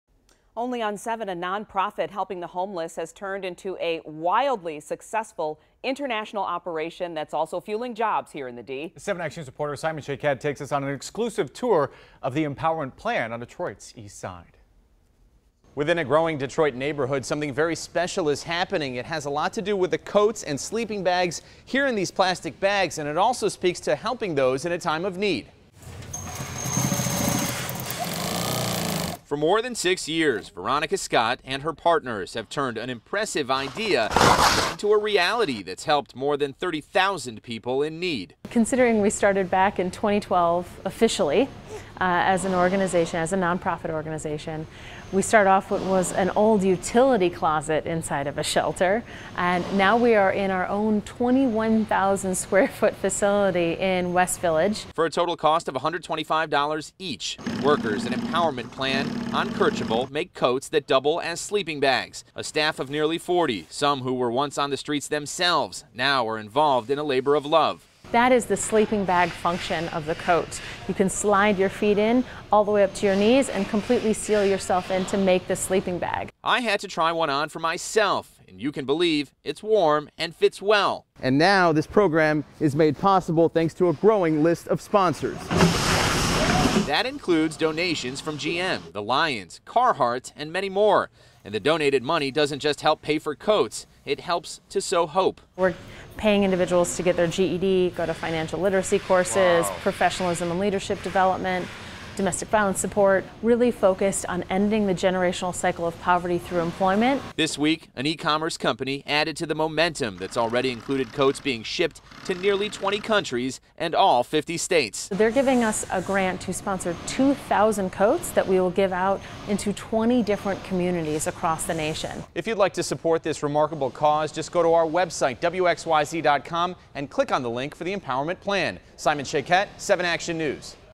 Detroit nonprofit makes coats that double as sleeping bags for the homeless
A nonprofit organization on Detroit's east side is committed to helping those in need and creating jobs here in the Motor City.